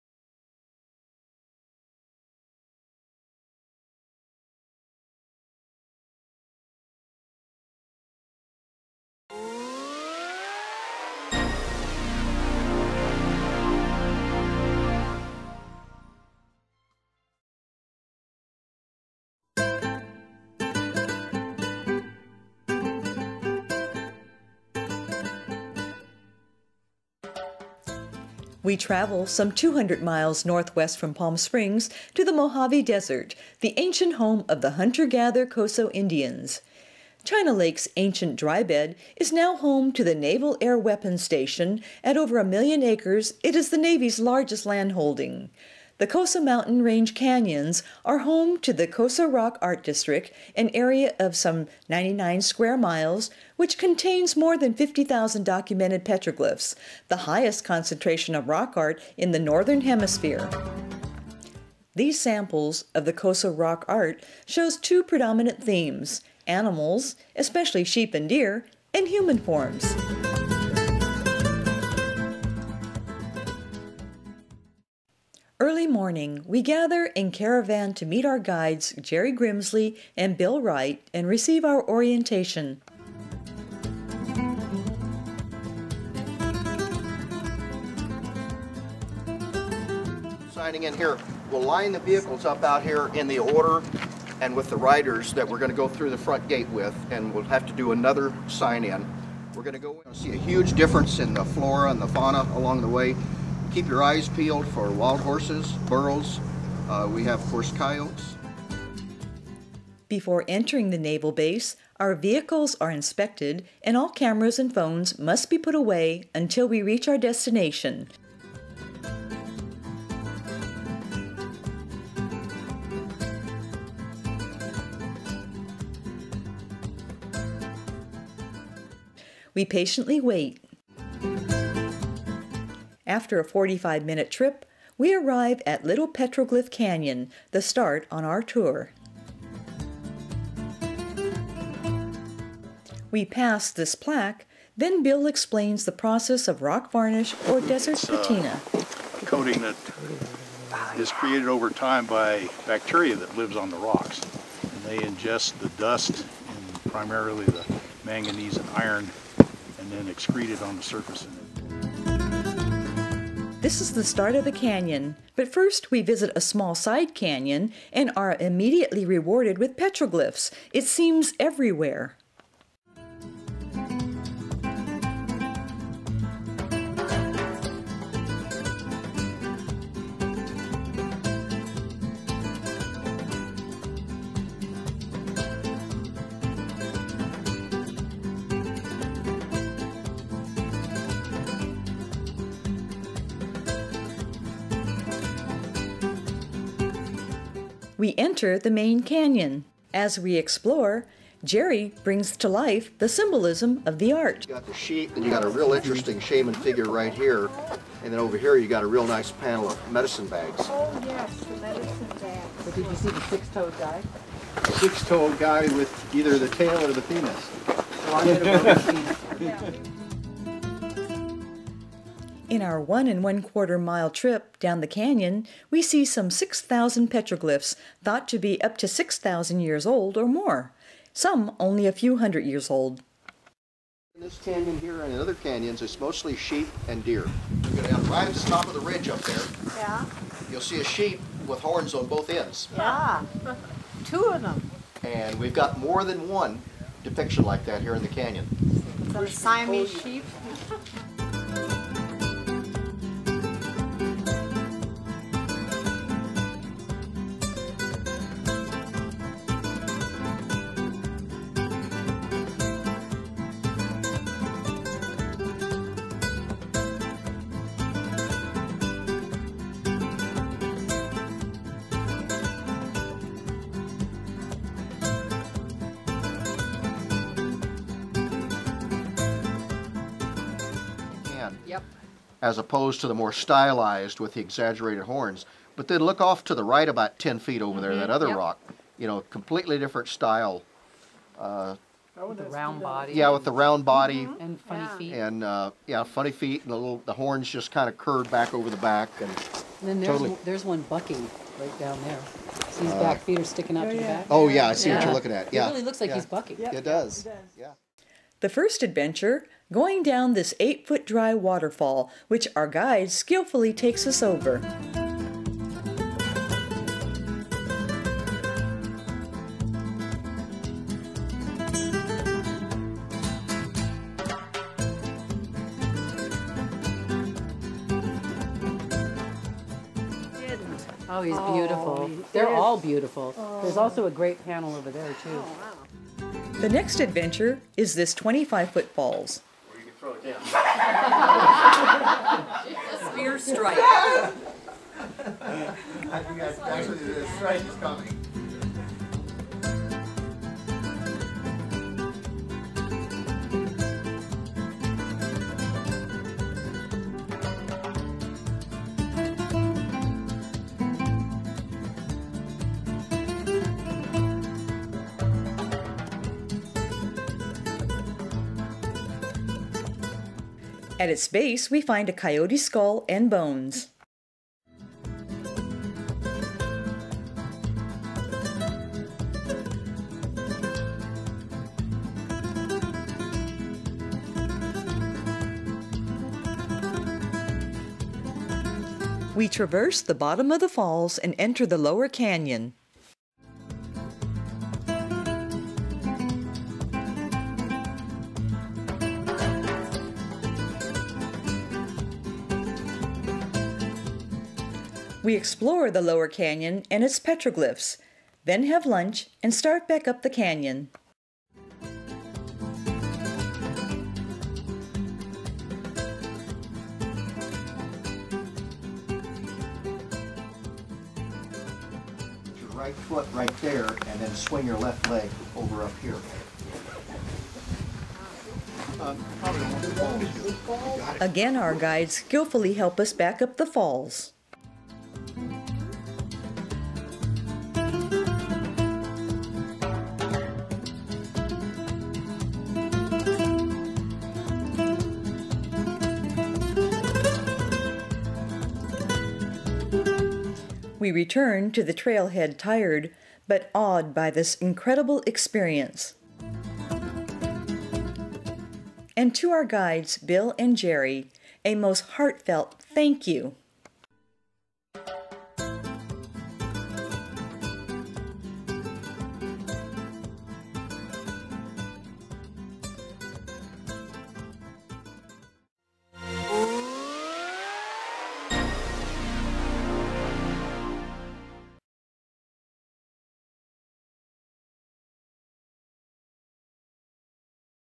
Little Petroglyph Canyon
Join the Sky Valley Hiking Club on this awesome visit to North Americas highest density of petroglyphs.  Our guides, Jerry and Bill, not only guide us but provide a rich commentary on this ancient art form.